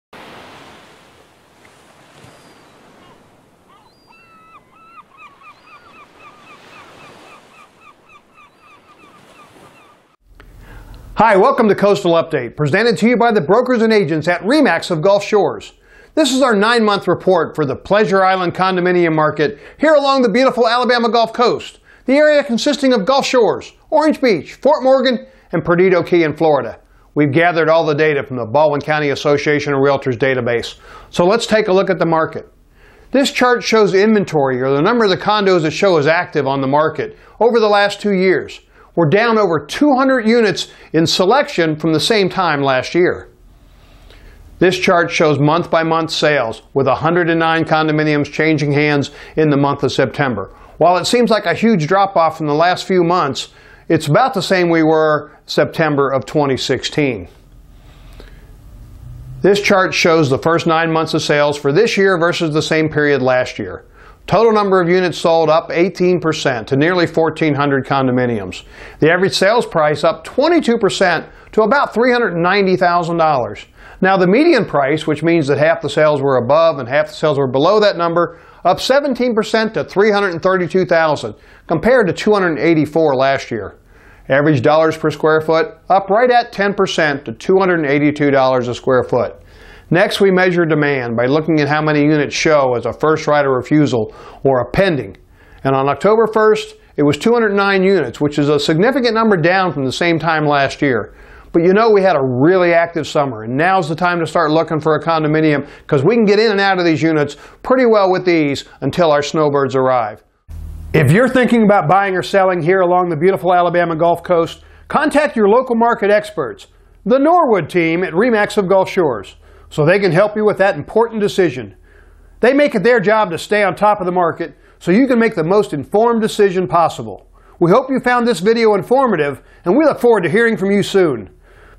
The Norwood Team: September Condo Market Update
The Norwood Team provides you with a look at the Condo Market for the first 9 months of 2017.